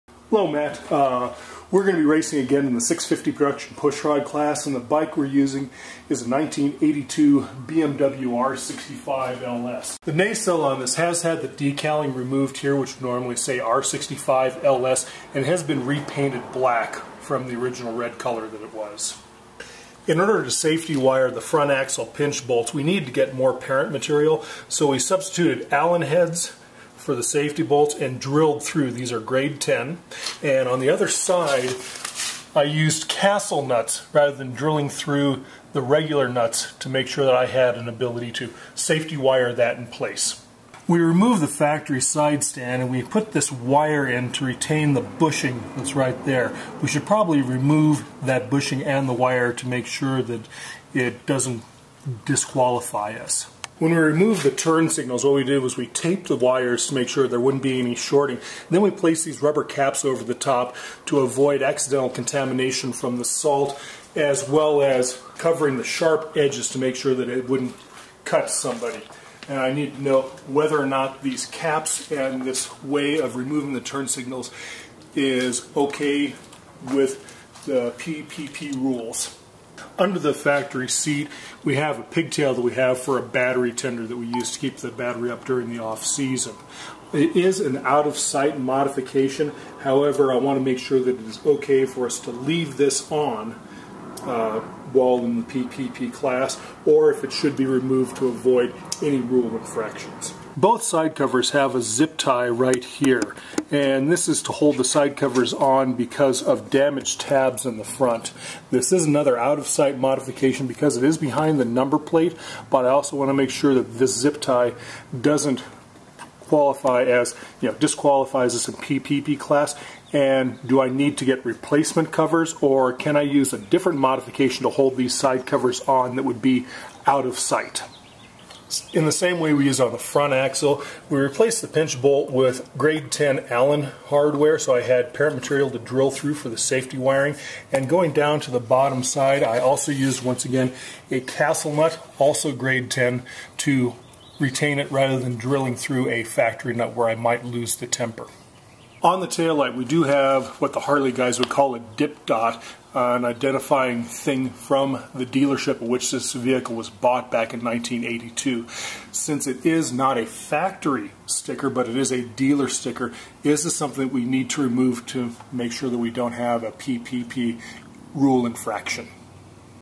Bonneville Safety1
Hello Matt,

I wanted to let you in on the safety changes we've made on our 1982 BMW R65LS.  We have the list below and a YouTube so you can see what we have done.  The reason I'd like you to take a look at this is that we want to avoid the problems we had last year with the camera on our 650cc P/PP entry.  Here is what has been changed from bone-stock for safety purposes:

Nacelle -- "R65-LS" sticker removed and nacelle repainted

Front Axel -- Pinch bolts replaced with grade 10.9 Allen heads and castle nuts -- This was to allow adequate material for safety wiring and drilling for wire holes.

Exhaust Crossover Clamps -- BMW only shows chrome replacements with same part number -- period literature appears to show painted hose clamps as seen here -- parts books do not have these -- should we go to chrome clamps?

Side Stand -- removed

Turn Signals -- All four removed and wires taped and tucked in -- rubber caps placed over stub ends because of potential salt contamination and safety from sharp edges.

Battery Tender Pigtail -- It is non-stock but it is out of sight -- Should this be removed to remain stock?

Side covers held on with zip ties due to damaged tabs -- out of sight -- is this OK, or is replacement or alternative attachment method needed?

Rear Axle -- Pinch bolt replaced with grade 10.9 Allen head and castle nut -- This was to allow adequate material for safety wiring and drilling for wire holes.

Tail light dip dot from dealer of purchase -- OK or remove?

Thanks for all that you do to help make Bonneville possible.

Take care,

Steven Farley and Paul Makarushka riding "Wagner's Flight" in the 650cc P/PP class.